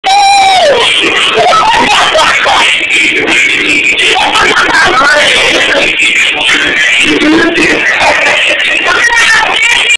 lap dance gone wrong
normas bday we had a party at hawthorne suites...she gets a lap dance from sidney and bryant...the turn out was interesting. lol. she almost passed out!